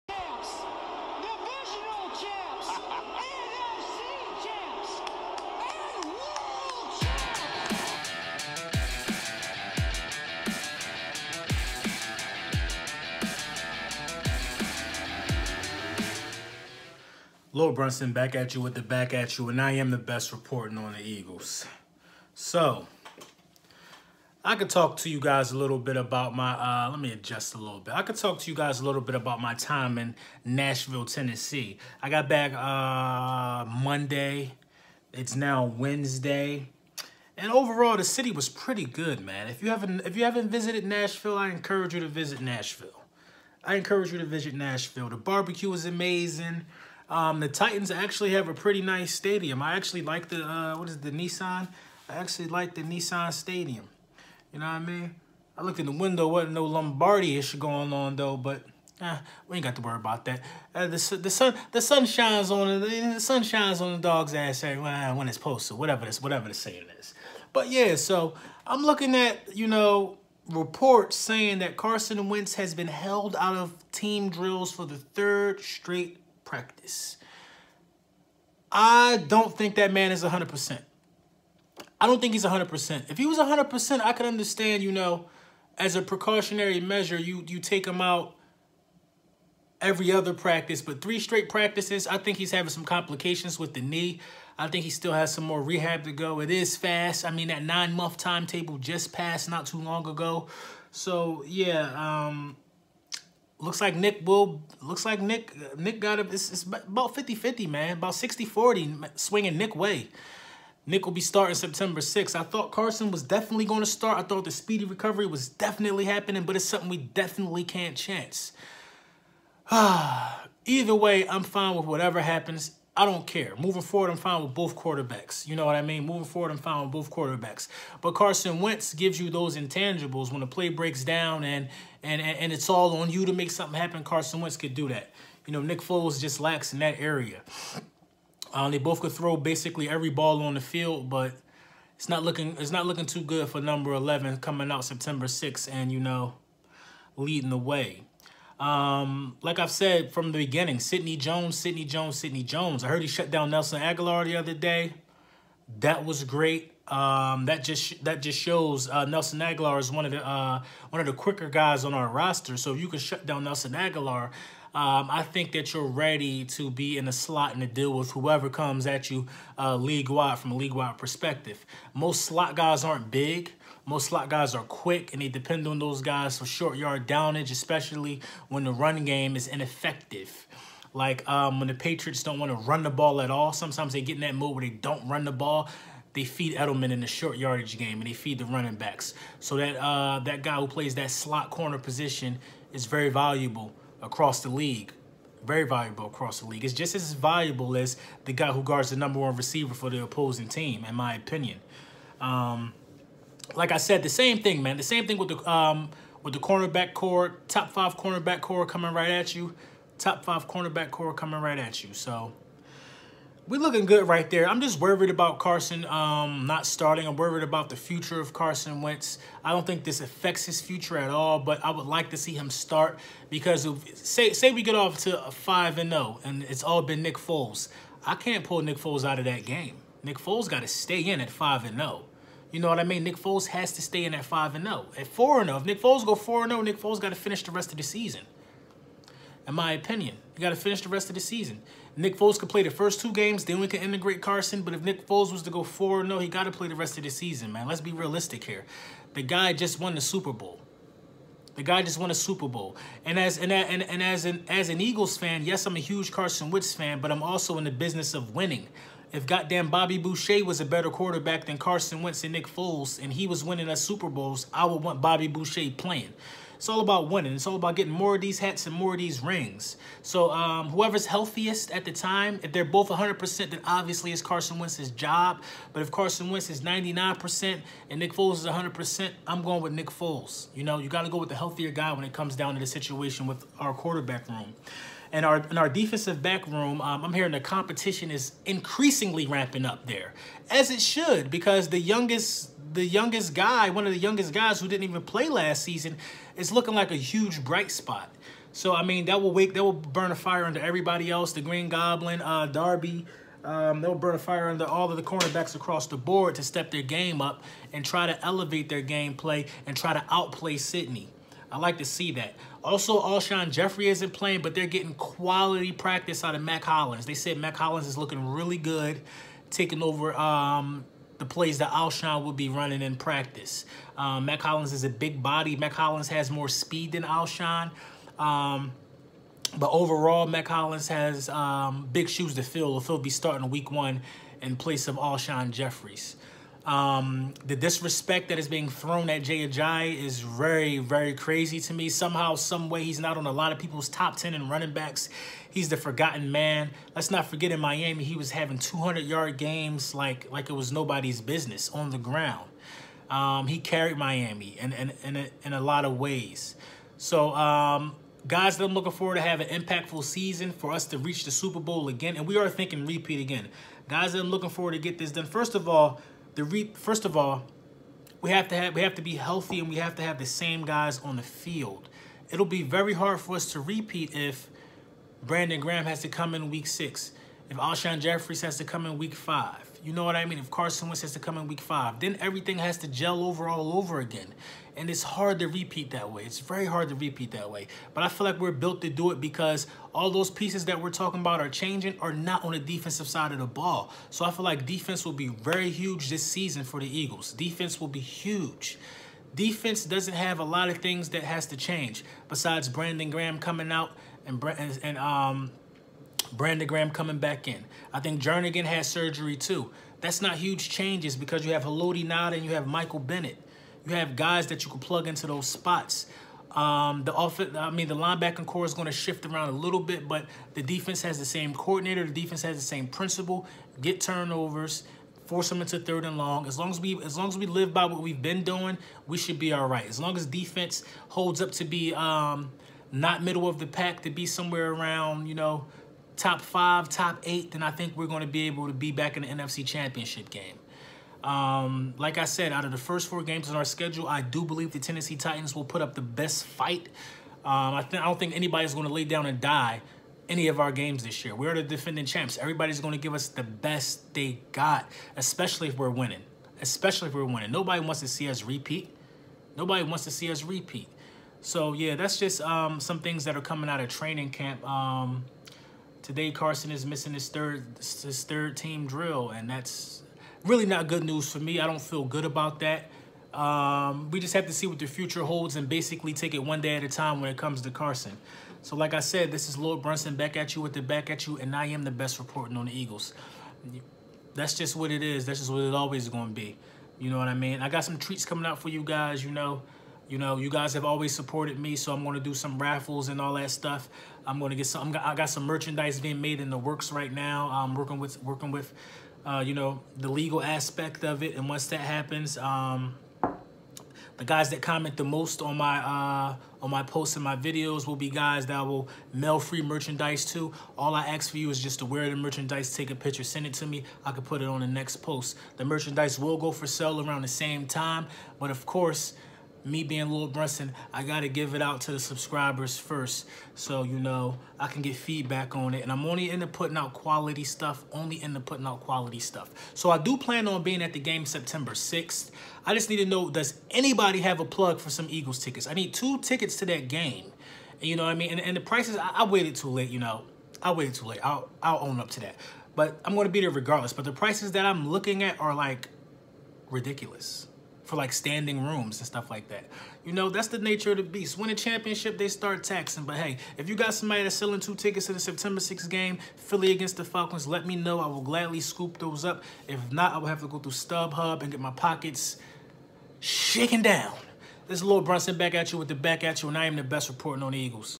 Carson Wentz isn’t as healthy as I thought.
Nick foles might be starting wether we like it or not.